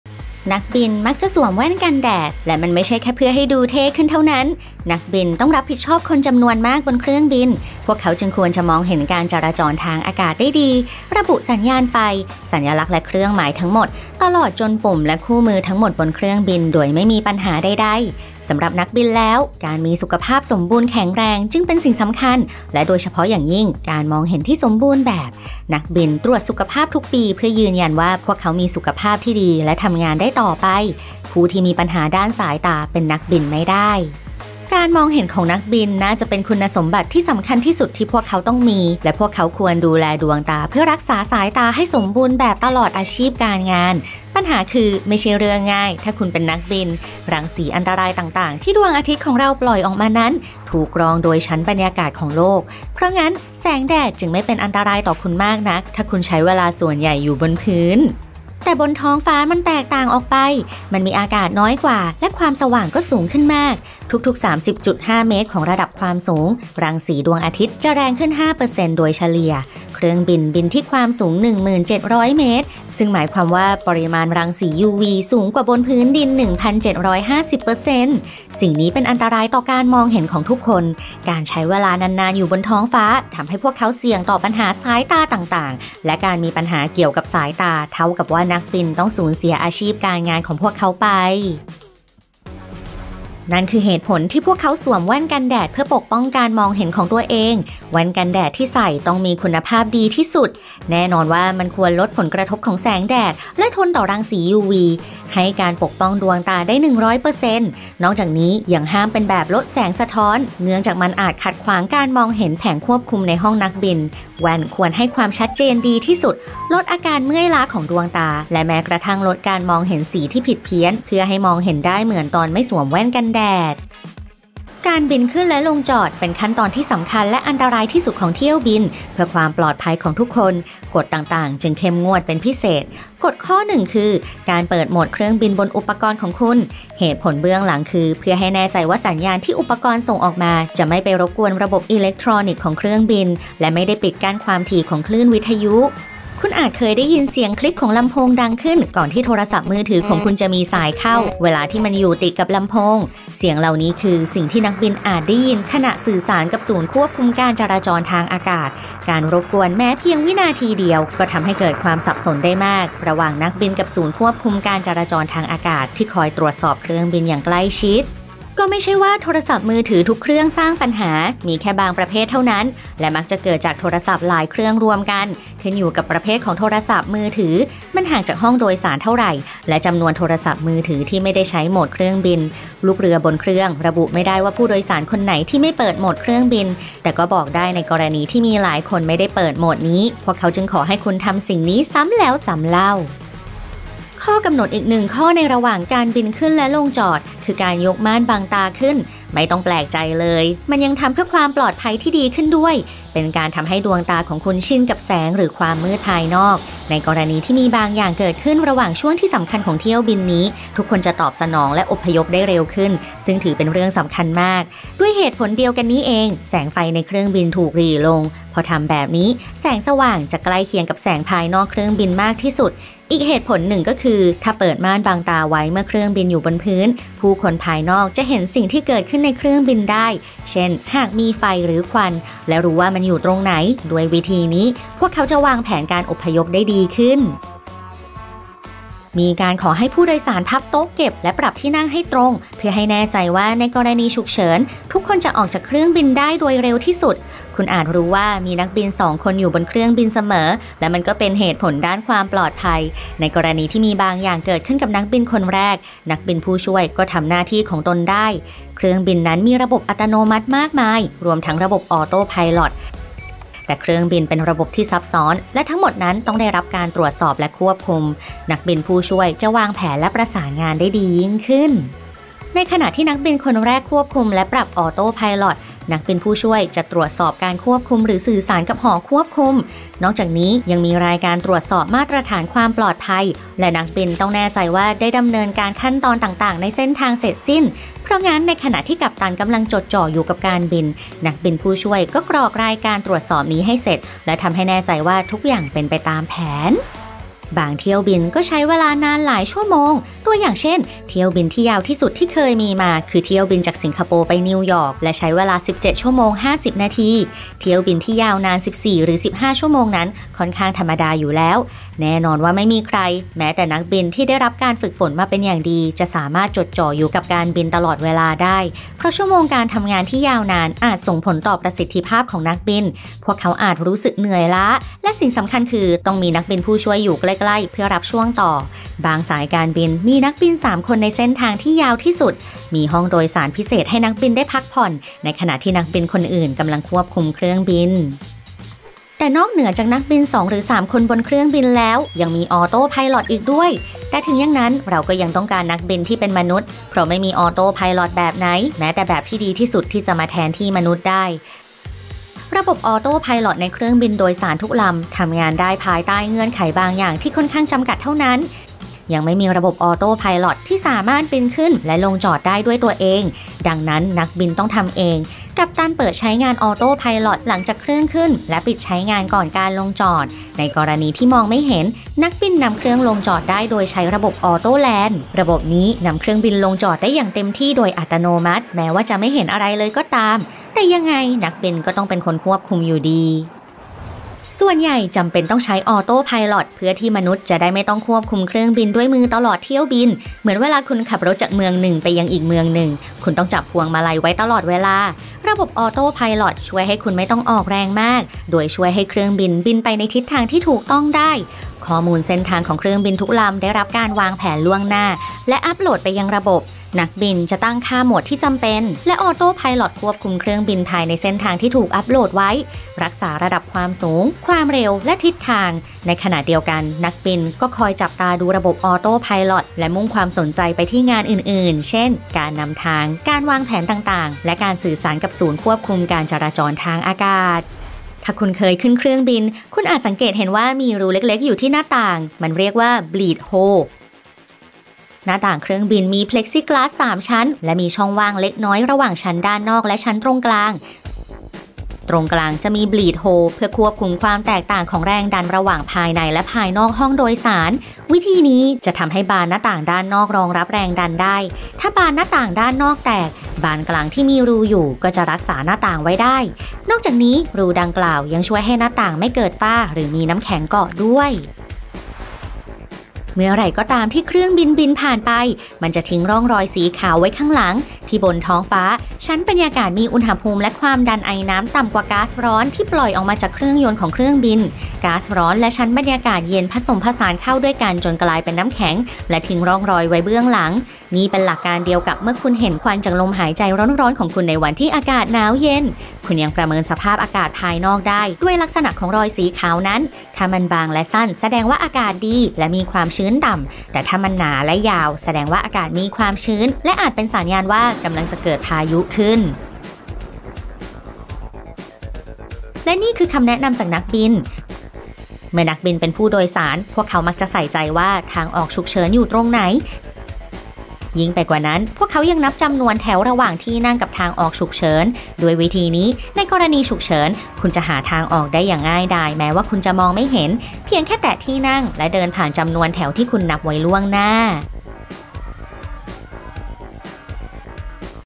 ทำไมนักบินสวมแว่นกันแดดเสมอ แต่ไม่ใช่แบบใดแบบหนึ่งโดยเฉพาะ
ถ้าคุณเคยเห็นภาพลูกเรือบนเครื่องบิน คุณอาจสงสัยว่าทำไมนักบินจึงสวมแว่นกันแดดอยู่เสมอ นักบินและแว่นกันแดดดูเหมือนจะไปด้วยกันเหมือนเครื่องบินและปีก พวกเขาแค่พยายามทำตัวให้ดูเท่หรือมีความหมายและจุดประสงค์บางอย่างหรือเปล่า มาตอบคำถามนี้และดูว่ามีแว่นกันแดดประเภทไหนบ้างที่ไม่อนุญาตให้นักบินใส่